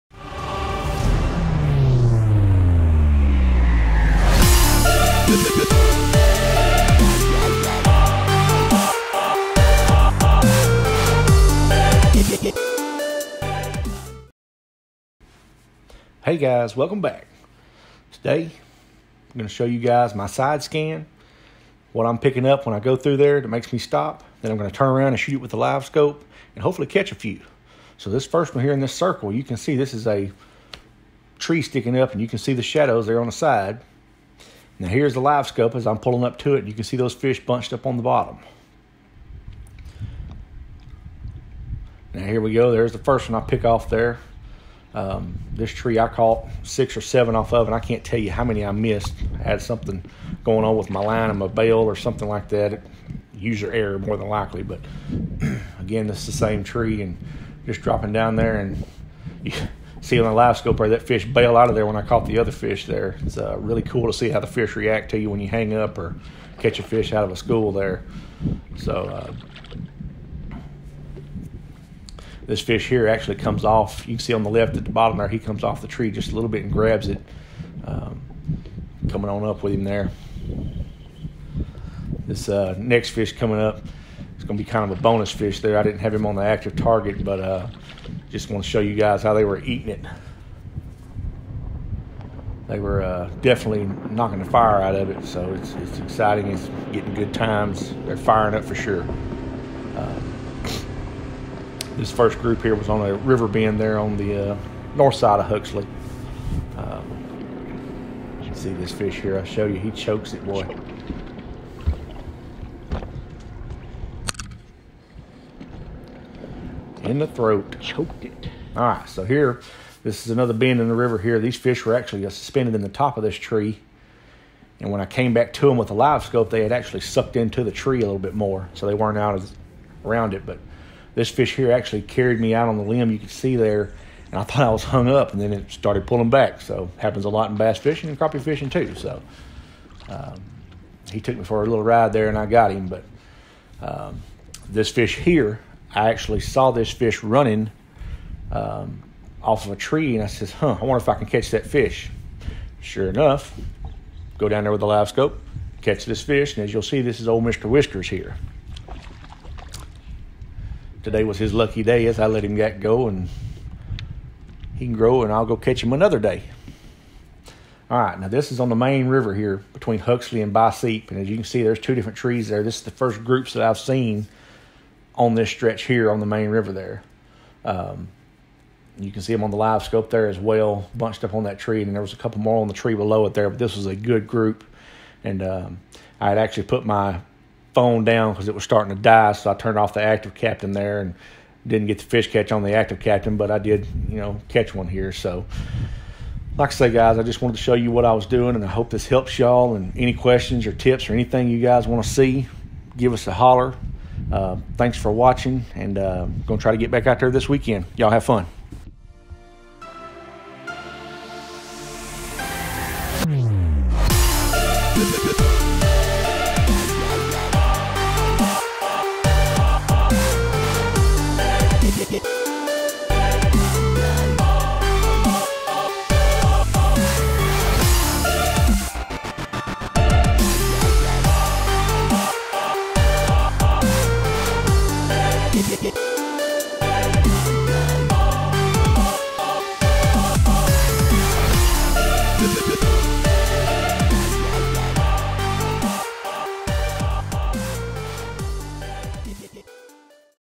SIDE SCANNING TO LIVESCOPING WITH ACTIVE CAPTAIN AND HUMMINBIRD
FROM SIDE SCANNING TO SEEING THE SAME IMAGE ON LIVESCOPE.  THIS VIDEO SHOWS YOU START TO FINISH WHAT TO LOOK FOR WHILE CHASING CRAPPIE ON TOLEDO BEND